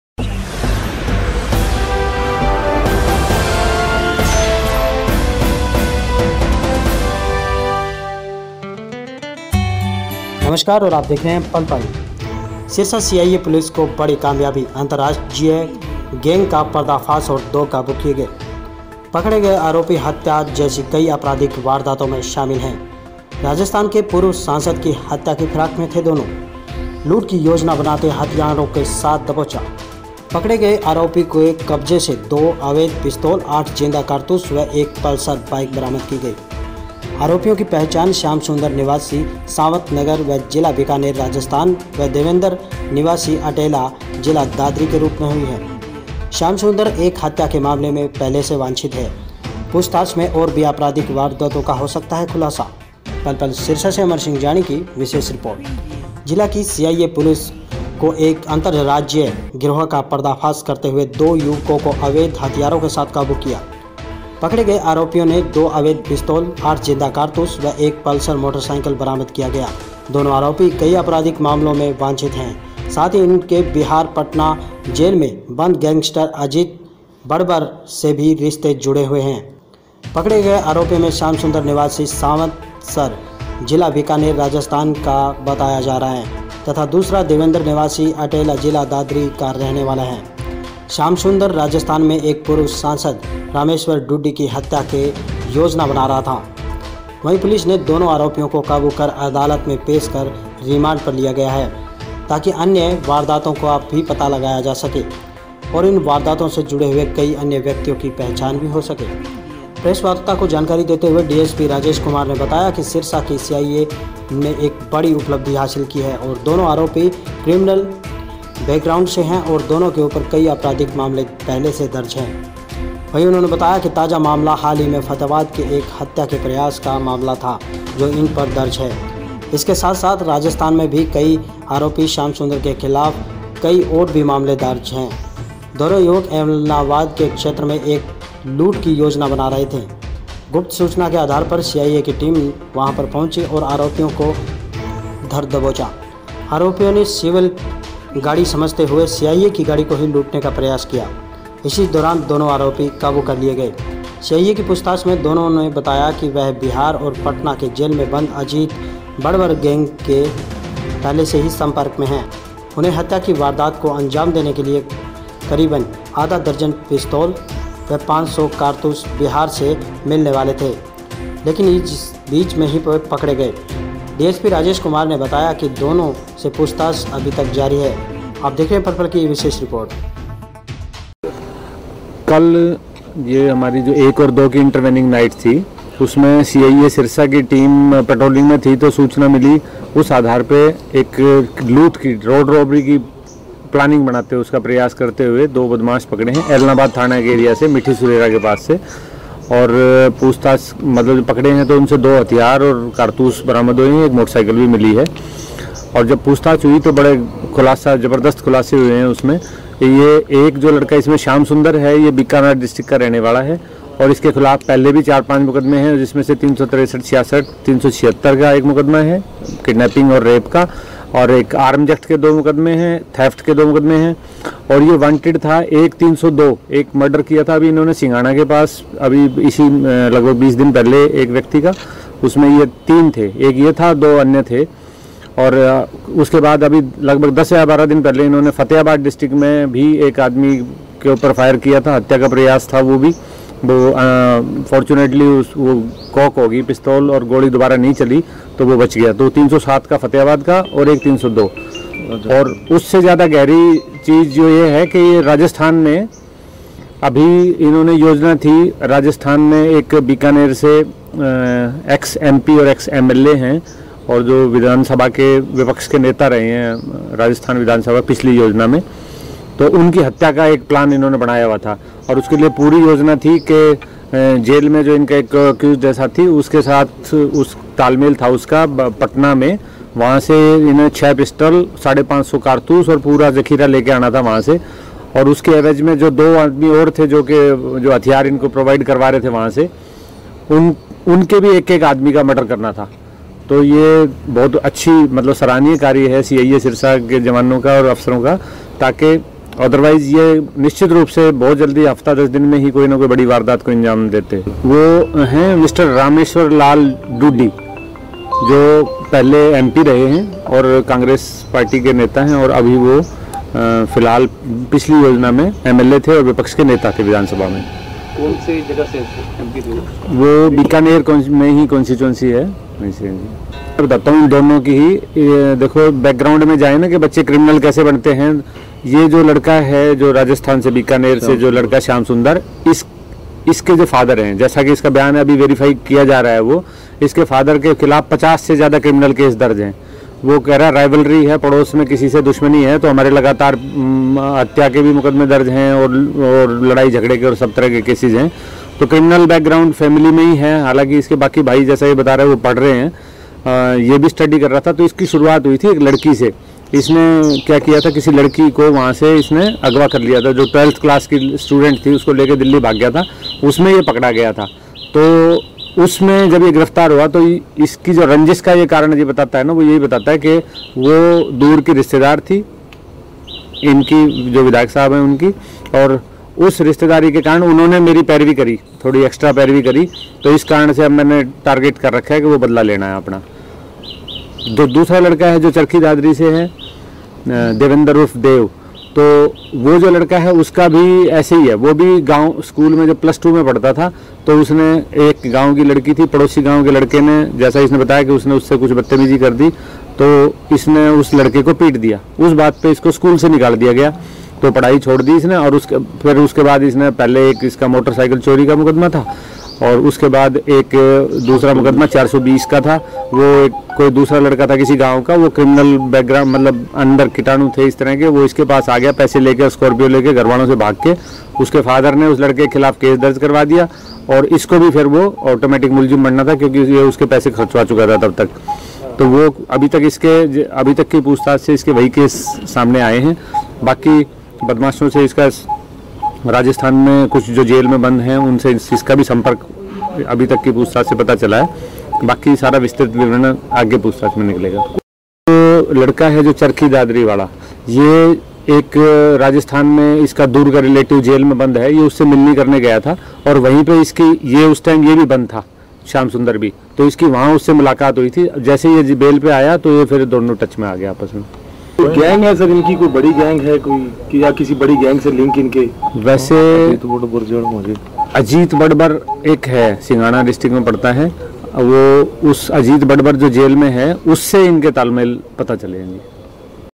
योजना बनाते हथियारों के साथ दबोचा-पूर्व सांसद की हत्या की फिराक में भी थे आरोपी
जिला की सीआईए सिरसा पुलिस ने एक अंतर राज्य गिरोह का पर्दाफाश करते हुए दो युवकों को अवैध हथियारों के साथ काबू किया है।  पकड़े गए आरोपियों से 2 अवैध पिस्तौल 8 जिंदा कारतूस व एक पलसर मोटरसाइकिल बरामद किया गया है।  दोनों आरोपी कई अपराधिक वारदातों में वांछित है।  साथ ही इनके बिहार के पटना जेल में बंद गैंगस्टर अजित बड़बर से भी जुड़े हुए बताए जा रहे हैं। पकड़े गए आरोपियों में श्याम सुंदर निवासी सांवतसर जिला बीकानेर राजस्थान का व दूसरा देवेंद्र निवासी अटेला जिला दादरी का रहने वाला है।  श्यामसुंदर राजस्थान के एक पूर्व सांसद रामेश्वर डूडी की हत्या के भी योजना बना रहा था।  पुलिस ने दोनों को काबू कर अदालत में पेश कर रिमांड हासिल किया जाएगा ताकि अन्य वारदातों का पता चल सके और इन वारदातों से जुड़े हुए किसी अन्य व्यक्ति की पहचान हो सके।